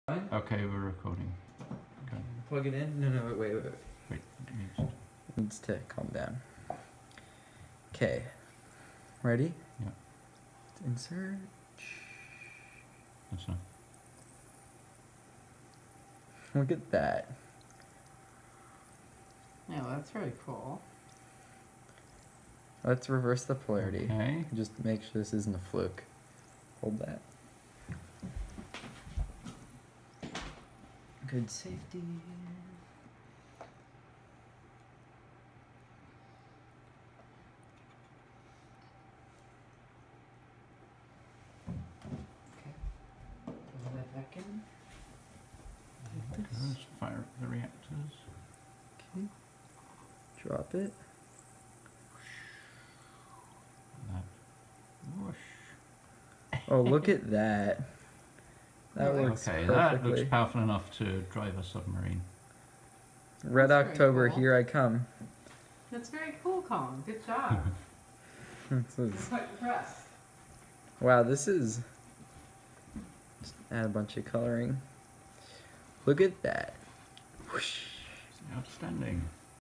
Magnetohydrodynamic drive demonstration
Built with a bunch of magnets, aluminum foil, and an old power supply. Inspired by Hunt for the Red October